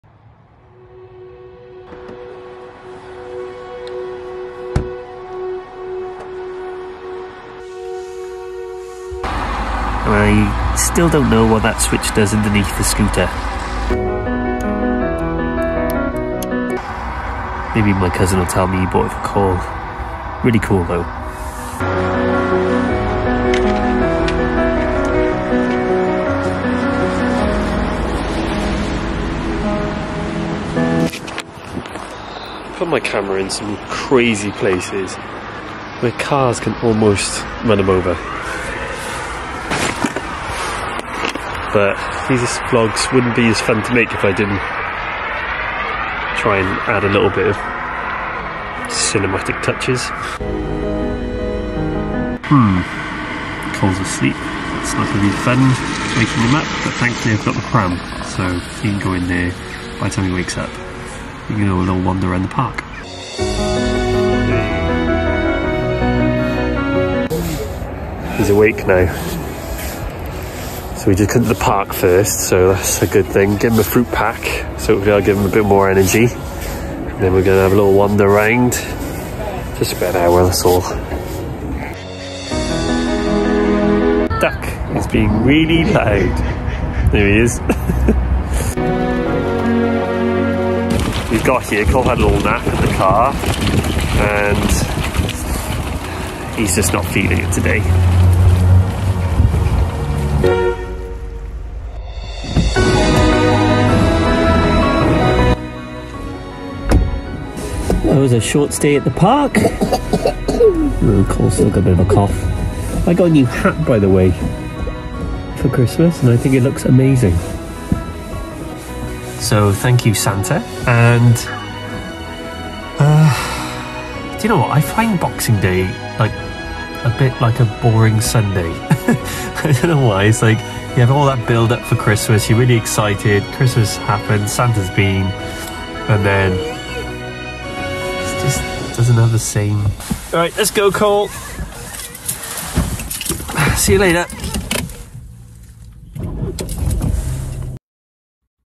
What is this switch?
What does this switch do on Coles scooter?

 I headed out to the park on Boxing Day. Cole had a nap on the way so I wasn’t sure how it was going to turn out. I tell you what I think of Boxing Day in general

Didn’t realise how loud ducks could be